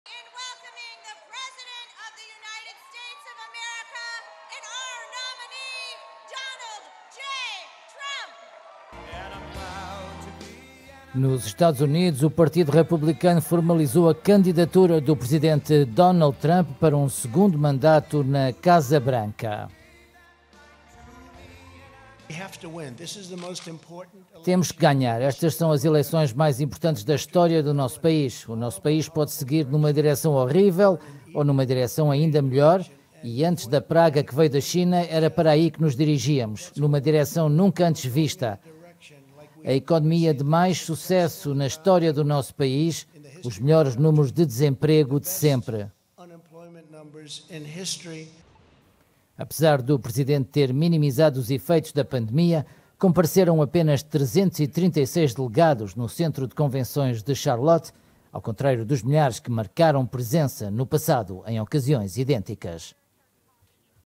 Donald Trump é oficialmente candidato
Convenção Republicana aponta o presidente à reeleição.